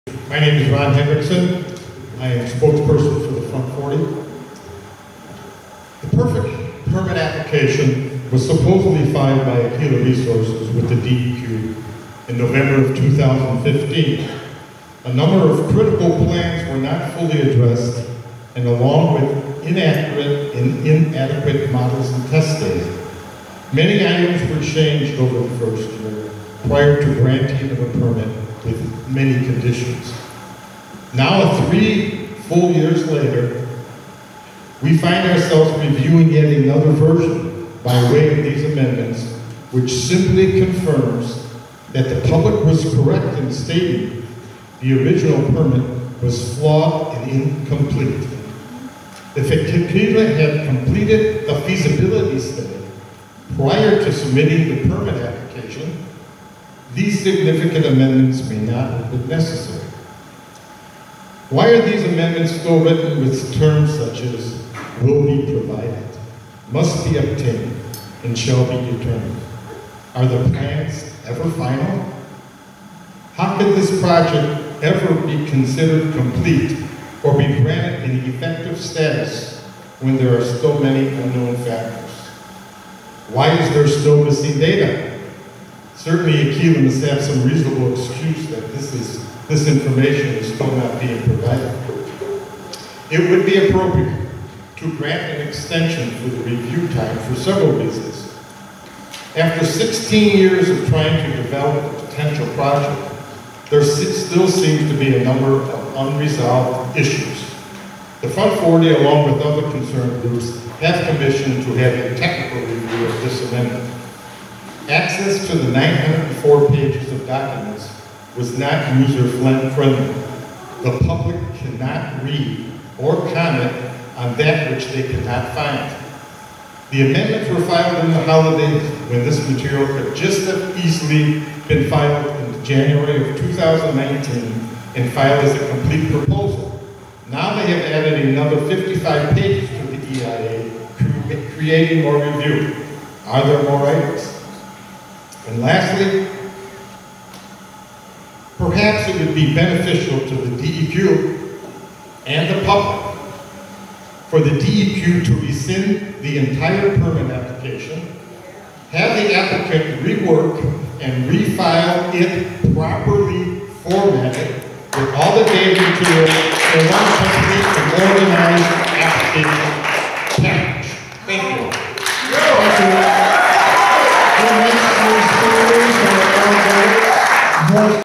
ron henriksen applic flaws
During the Jan. 9, 2019, Department of Environmental Quality hearing on Aquila's proposed Back 40 Part 632 mining permit amendment, Ron Henriksen, spokesperson for the Front 40 environmental group, challenges the DEQ on missing data in the 2015 permit and in the project as a whole. (Video by Keweenaw Now)